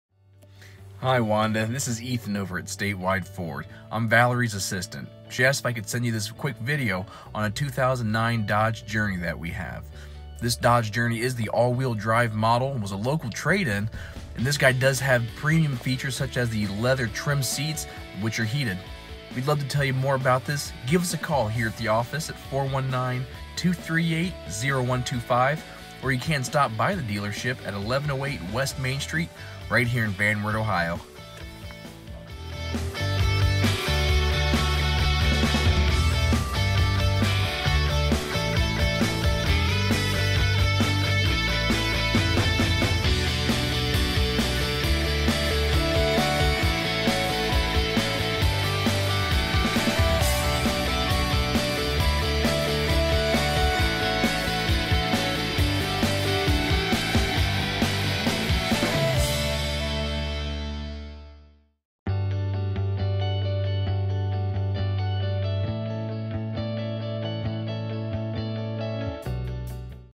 Dodge Journey Here At Statewide Ford Lincoln In Van Wert, OH
Ethan introducing himself and the 2009 Dodge Journey here at Statewide Ford Lincoln.